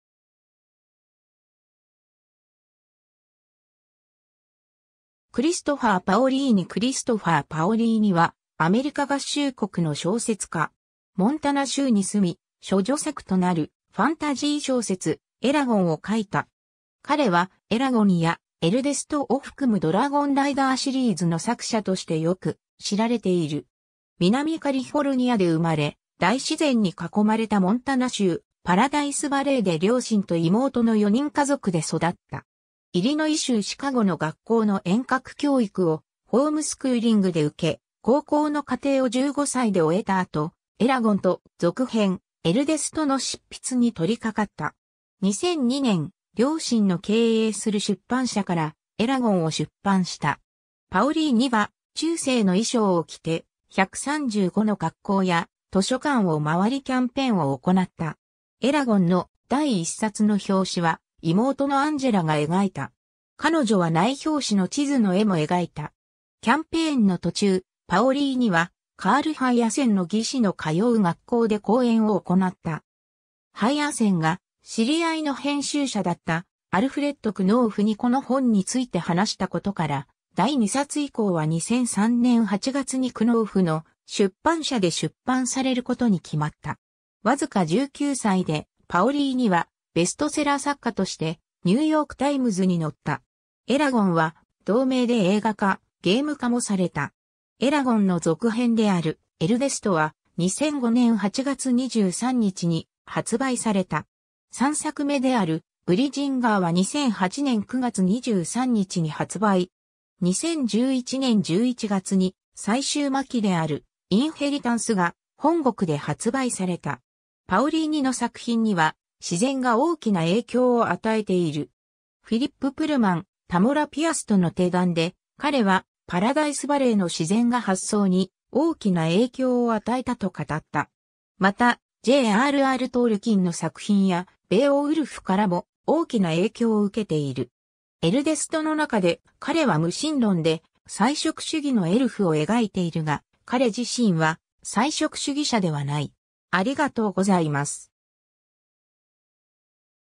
クリストファー・パオリーニ
Christopher Paolini クリストファー・パオリーニ（Christopher Paolini、1983年11月17日 - ）はアメリカ合衆国の小説家。
モンタナ州に住み、処女作となるファンタジー小説『エラゴン』を書いた。
彼は『エラゴン』や『エルデスト』を含むドラゴンライダーシリーズの作者としてよく知られている。
 南カリフォルニアで生まれ、大自然に囲まれたモンタナ州パラダイスバレーで両親と妹の4人家族で育った。
イリノイ州シカゴの学校の遠隔教育をホームスクーリングで受け、高校の課程を15歳で終えたあと『エラゴン』と続編『エルデスト』の執筆に取り掛かった。
 2002年、両親の経営する出版社から『エラゴン』を出版した。
パオリーニは中世の衣装を着て、135の学校や図書館を回りキャンペーンを行った。
『エラゴン』の第1刷の表紙は妹のアンジェラが描いた。
彼女は内表紙の地図の絵も描いた。
 キャンペーンの途中、パオリーニはカール・ハイアセンの義子の通う学校で講演を行った。
ハイアセンが知り合いの編集者だったAlfred Knopfにこの本について話したことから、第2刷以降は2003年8月にKnopfの出版社で出版されることに決まった。
わずか19歳で、パオリーニはベストセラー作家としてニューヨーク・タイムズに載った。
『エラゴン』は同名で映画化・ゲーム化もされた。
 『エラゴン』の続編である『エルデスト』は、2005年8月23日に発売された。
3作目である『ブリジンガー』は2008年9月23日に発売。
2011年11月に最終巻である『インヘリタンス』が本国で発売された。
 パオリーニの作品には自然が大きな影響を与えている。
フィリップ・プルマン、タモラ・ピアスとの鼎談で、彼はパラダイスバレーの自然が発想に大きな影響を与えたと語った。
またJ・R・R・トールキンの作品や『ベーオウルフ』からも大きな影響を受けている。
 『エルデスト』の中で、彼は無神論で菜食主義のエルフを描いているが、彼自身は菜食主義者ではない。